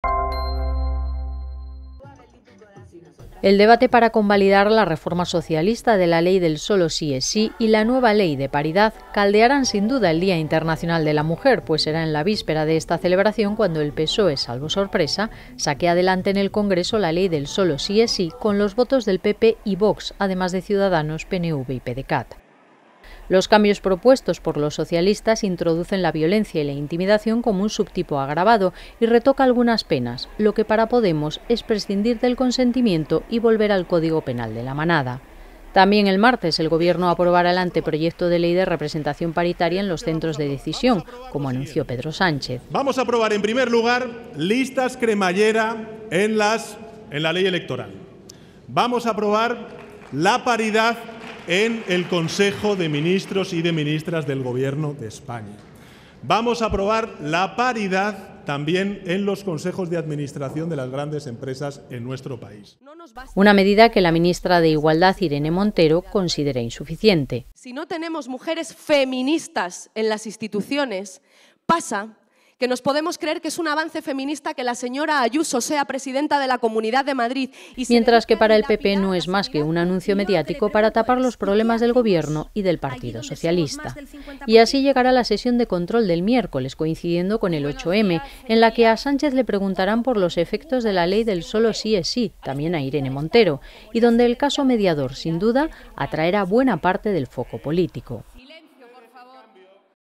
La reforma del sólo sí es sí y la nueva ley de paridad copan la semana del 8M
Madrid, 6 mar (EFE).- El debate para convalidar la reforma socialista de la ley del sólo sí es sí, con el previsible voto en contra de Unidas Podemos, y la nueva ley de paridad que aprobará el Gobierno centran la semana política del 8M, en la que también se pedirán explicaciones al PSOE por el caso Mediador.
Todo esto sucederá justo en la víspera de la celebración del Día Internacional de la Mujer, que los dos socios de gobierno celebran con grandes discrepancias sobre todo a cuenta de los cambios que ha propuesto la parte socialista del Ejecutivo a la ley de Irene Montero para atajar las rebajas de penas a agresores sexuales que se han producido en algunos casos.
Edición y locución: Clara González.
VÍDEO CON IMÁGENES DE TERCEROS. CONSULTAR RESTRICCIONES EN REDES.